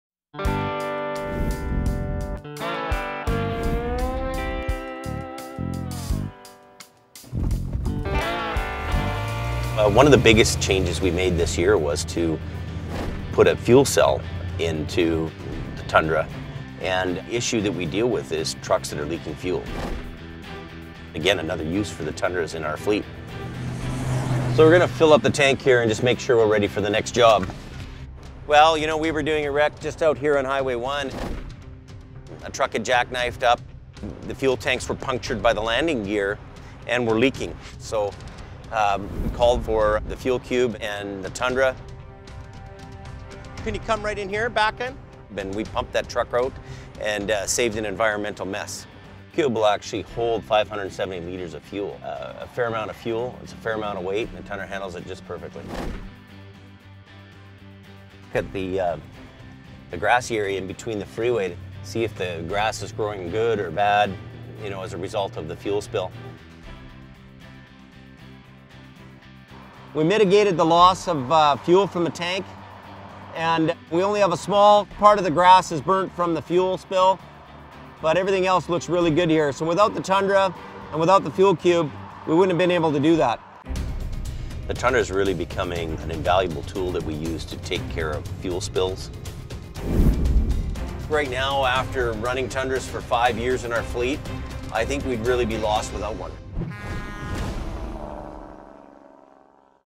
A New Tool For Jamie Davis Fleet
Jamie outfits a Tundra with a fuel transfer tank.